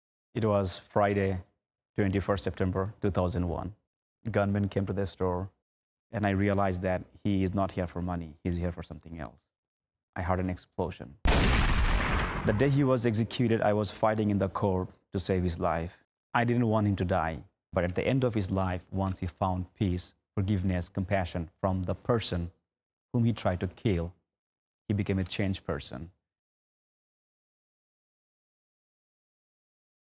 PSA Rais Bhuiyan 2
This is a PSA that I did for ICNA and is currently airing on TV.

If someone shot you in the face and left you for dead, would you try to save his life?

Well, this is the story of Rais Bhuiyan, a Muslim Immigrant from Bangladesh, who despite having thirty-eight pellets in his face and being blind in his right eye, has campaigned for months to persuade the state of Texas to spare the life of his shooter, Mark Stroman who shot him for no reason other than the fact that he is a Muslim. Unfortunately Mark Stroman was executed on July 20th, 2011 but he did as a changed man. Mark's last words were "Hate is going on in this world and it has to stop, Hate causes a lifetime of pain."

This PSA was Produced by ICNA BayArea in association with Under Da Hood Films.
Directed by Hisham Izzeldin
Exec Production by Micheal Farnia
Art and set Design by Isa Magomedov
AD Marcelle Kube